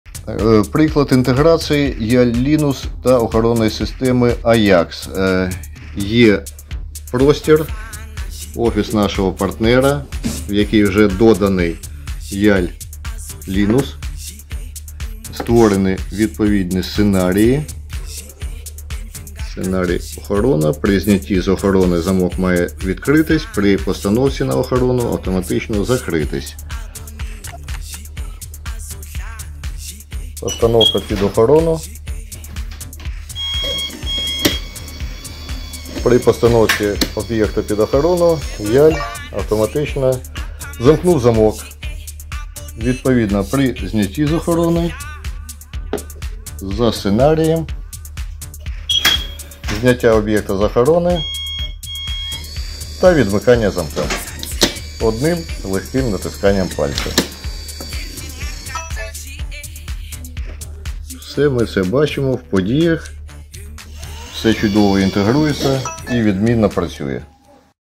Yale Linus + Ajax
Вітаємо користувачів AJAX.

На ринку безпеки відбулась дуже значна подія: компанії Yale (один з глобальних брендів групи ASSA ABLOY, який виробляє такі розумні замки, як Yale Linus Smart Locks). та Ajax Systems (відомі системи безпеки вітчизняного виробництва AJAX) оголосили про завершення процесу інтеграції двох систем.

Відтепер в користувачів системою безпеки Ajax (Аякс) є унікальна можливість керувати розумним замком Yale Linus безпосередньо з додатку Ajax Secur.

Інтеграція не потребує ніякого додаткового обладнання або модифікації наявного – вона реалізована за принципом С2С (Cloud-to-Cloud) – тобто взаємодія двох систем здійснюється на рівні відповідних хмарних сховищ.

Все, що необхідно для інтеграції розумного замка Yale Linus до наявної системи Ajax – мати встановлений на смартфоні додаток Ajax Security System (останньої версії 3.0), та розумний замок Yale Linus якій через Wi-Fi брідж підключений до мережі  та має обліковий запис в додатку YALE Home.

Інтеграція додає користувачеві нові зручні можливості, насамперед – керувати своєю охоронною системою Ajax та розумним замком Yale Linus з одного додатку, наприклад, при поверненні додому зняти свою оселю з під охорони та відімкнути замок одним натисканням кнопки в додатку й навпаки, при постановці системи під охорону – автоматично замкнути й замок.

Придбати розумний замок Yale Linus можна на онлайн вітрині lock.ua
Якщо у вас виникли питання стосовно нашої продукції.
Або у вас є пропозиції що до співпраці.
☎️ Телефонуйте нам на номер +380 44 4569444
📧 Або пишіть на єлектронні адреси: